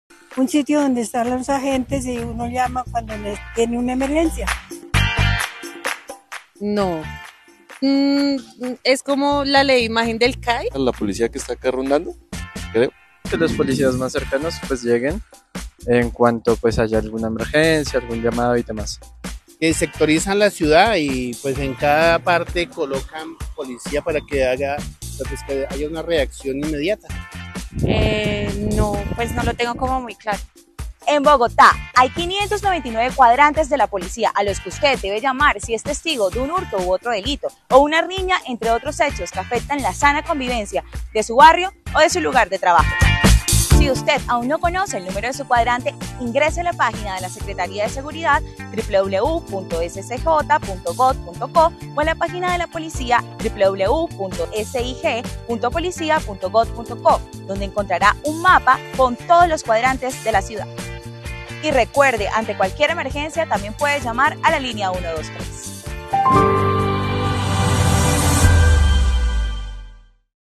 Cuadrantes de Policía en Bogotá: identifica el de tu barrio y comunidad
La Policía de Bogotá cuenta con 599 cuadrantes, donde la ciudadanía puede reportar delitos, faltas de convivencia y solicitar acompañamientos.